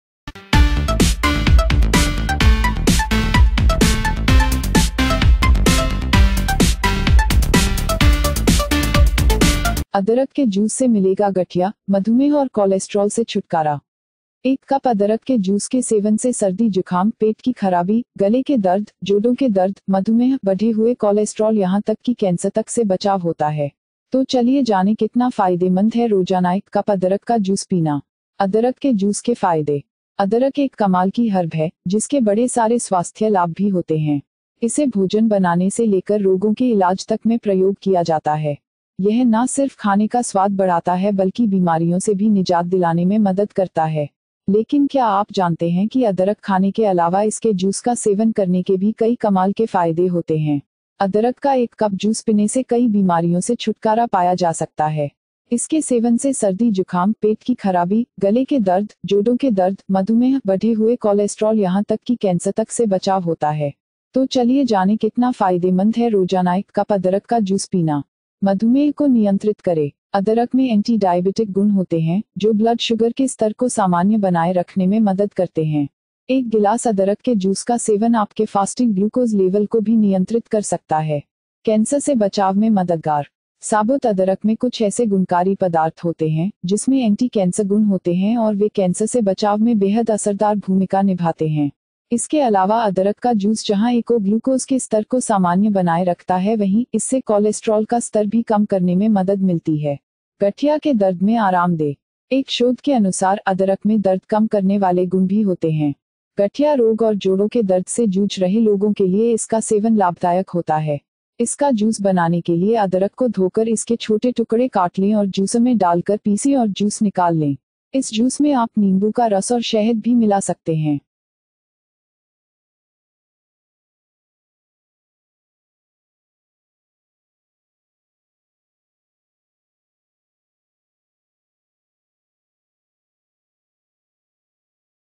अदरक के 8 फायदे जानकर दंग रह जायेंगे आप │ Health Benefits of Ginger │Adrak Ke Fayde
अदरक के जूस से मिलेगा गठिया, मधुमेह और कोलेस्‍ट्रॉल से छुटकारा
एक कप अदरक के जूस के सेवन से सर्दी जुखाम, पेट की खराबी, गले के दर्द, जोडों के दर्द, मधुमेह, बढे हुए कोलेस्‍ट्रॉल यहां तक कि कैंसर तक से बचाव होता है। तो चलिए जानें कितना फायदेमंद है रोजानाएक कप अदरक का जूस पीना।